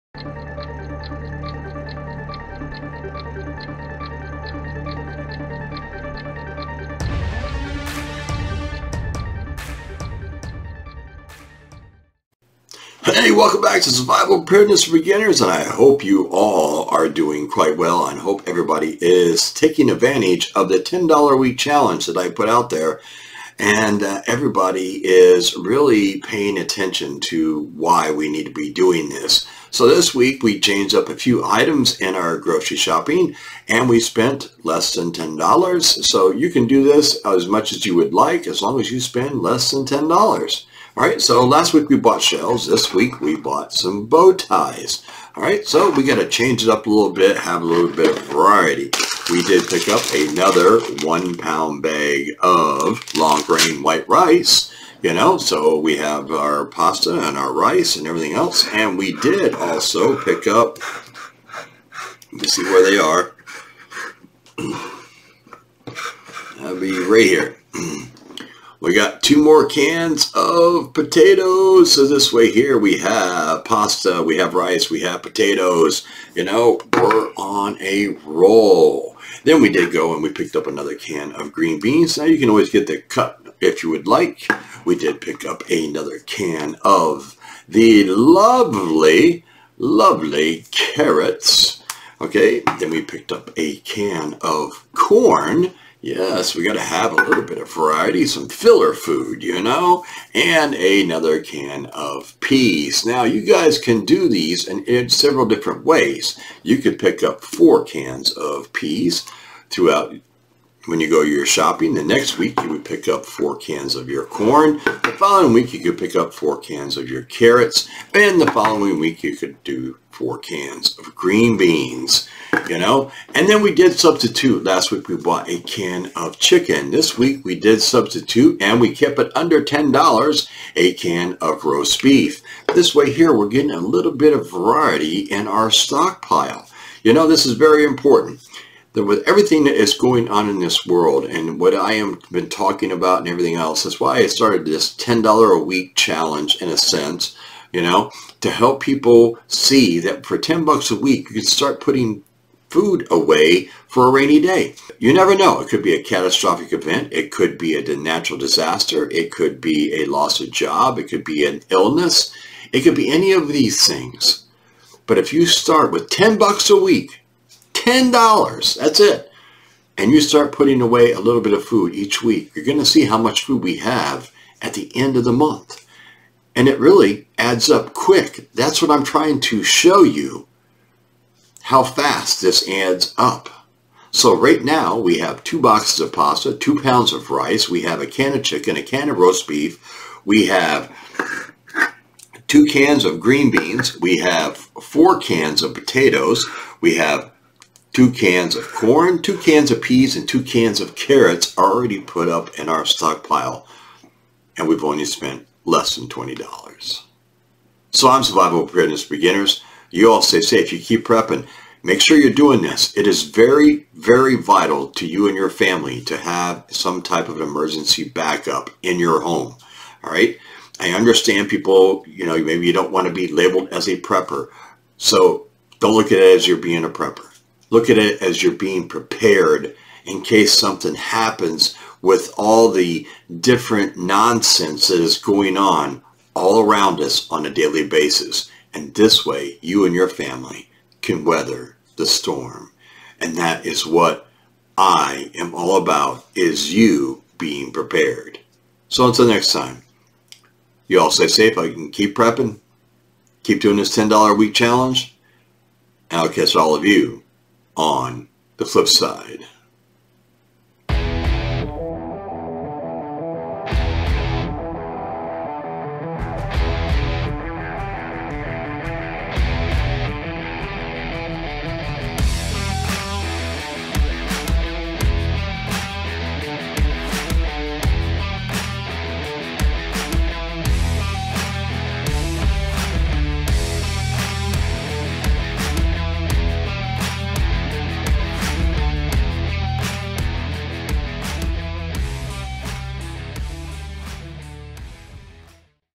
Food Stockpile Week 2
A food stockpile is a collection of non-perishable food items that are stored for future use. This can include canned goods, dried goods, and other items that have a long shelf life. Stockpiling food can be done for a variety of reasons, such as preparing for an emergency or natural disaster, or to take advantage of bulk buying discounts. It is important to regularly rotate items in a food stockpile. Additionally, it's important to store the food in a cool, dry place to maintain freshness.

FoodSaver FM5200 2-in-1 Automatic Vacuum Sealer